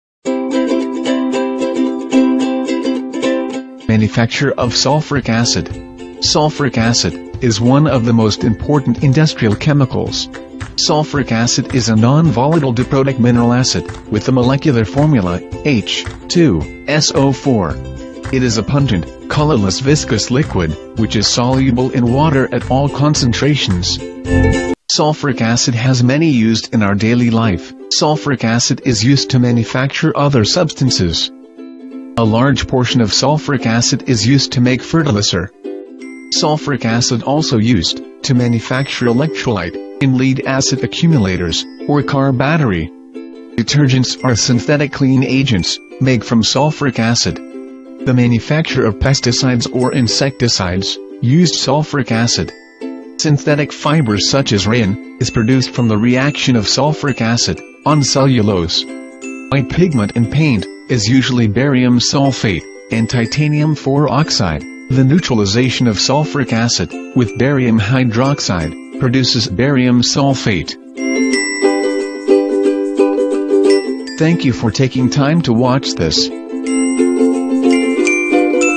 Uses of sulphuric acid
SPM - Chemistry - Form 4
Chapter 9 : Manufactured Substances in Industry
9.1 Manufacture of sulphuric acid